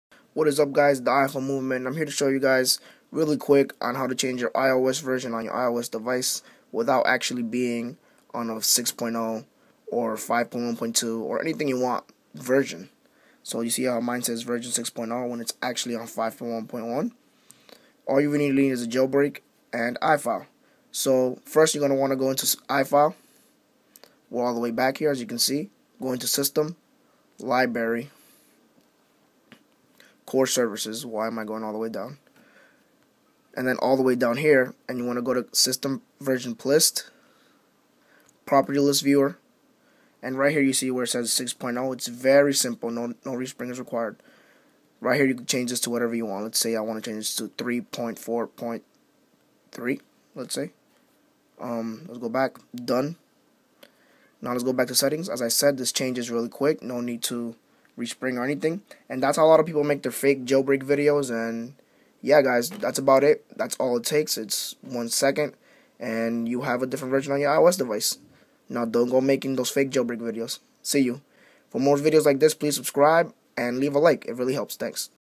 How To Change The iOS Version on Your iDevice
In this turotial I show you how to change the iOS Version on your iDevice.

No this will not mess up your device, but make sure to change it back to the original version because when you try to download tweaks most of the time it will not work.

Things you need:

An iDevice
Cydia
iFile